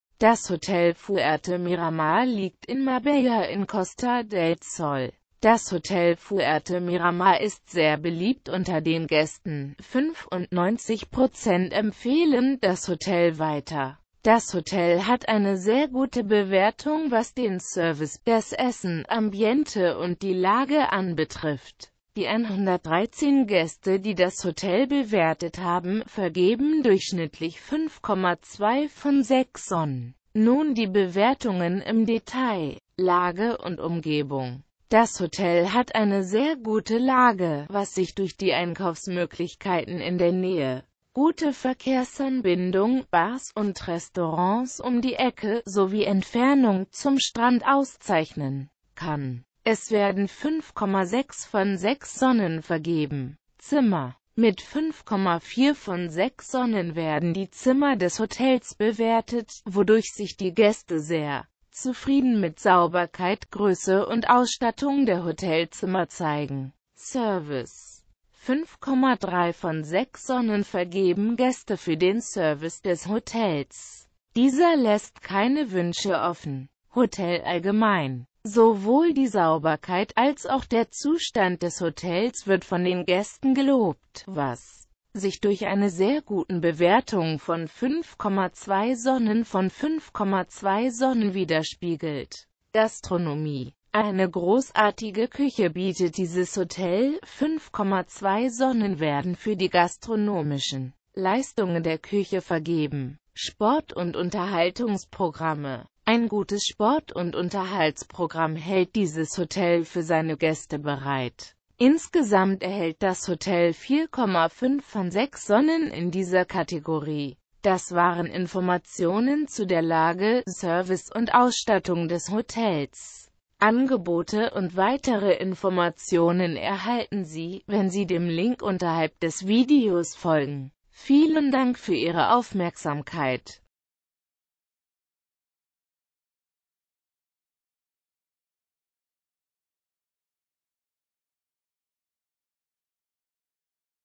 Hotel Fuerte Miramar in Marbella (Costa del Sol - Spanien) Bewertung
Das Hotel Fuerte Miramar liegt in der Stadt Marbella welche zu Spanien (Europa) gehoert.

 Das Hotel Fuerte Miramar ist sehr beliebt unter den Gaesten, 95% empfehlen das Hotel weiter. Zudem erhaelt das Hotel 5.2 von 6 Sonnen.
Lage und Umgebung:
Zimmer:
Service:
 5,3 von 6 Sonnen vergeben Gaeste fuer den Service des Hotels. Dieser laesst keine Wuensche offen.

Informationen zur Verfuegbarkeit von Transfer zum Hotel, WLAN in den Zimmern des Hotel Fuerte Miramar, sowie Details zum Animations-Progamm, Entfernung zum Strand oder zu Diskotheken erhalten Sie bei Ihren Reiseveranstalter oder auf der Webseite des Hotels. Neben den hier verwendeten Bewertungen, lohnt sich ein Blick auf die ggf. vorhandene Hotel Fuerte Miramar Facebook-Seite.

Quellenhinweis:
Die verwendeten Informationen stammen von HolidayCheck.de. Sie wurden am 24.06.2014 abgerufen. In diesem Video wurden Bilder aus Marbella oder allgemeine Bilder aus Spanien verwendet. Die gezeigten Orte und Landschaften dienen als Beispiel und muessen nicht unbedingt in der Naehe des Hotels liegen.